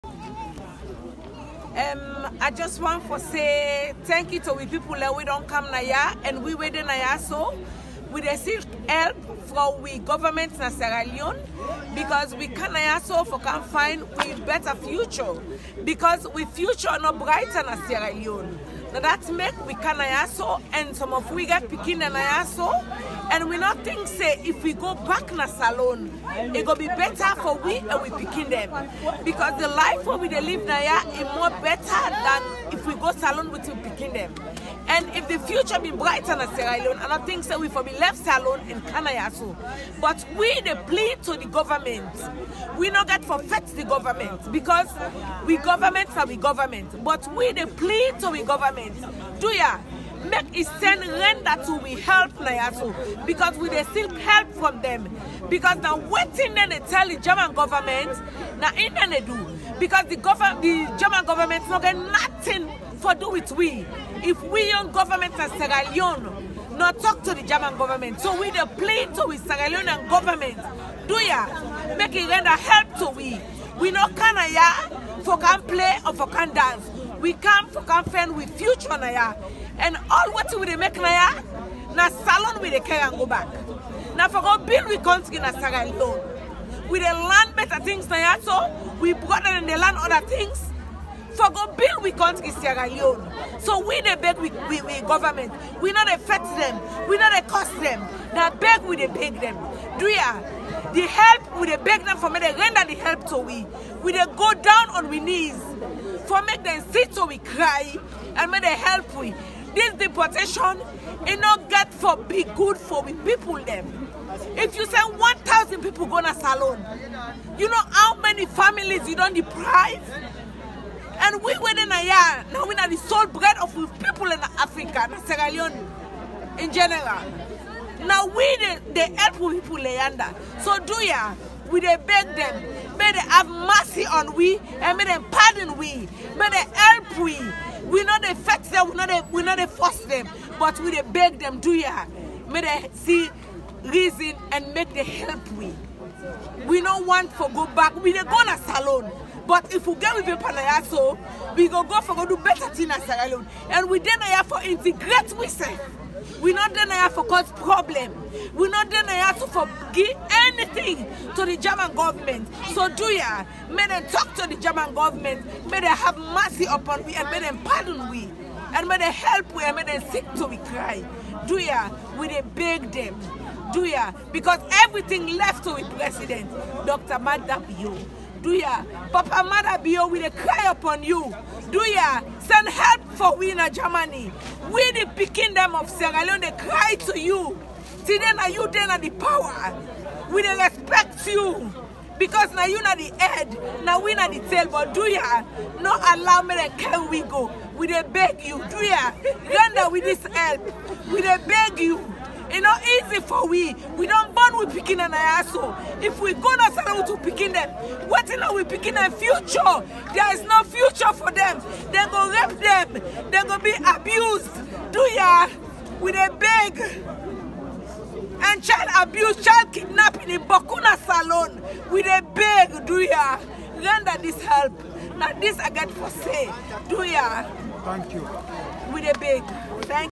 Sierra Leone Refugees against Deportation in Munich, October 2021, Camp Hofmannstraße - Video 1
Enthält Schilderungen von Gewalt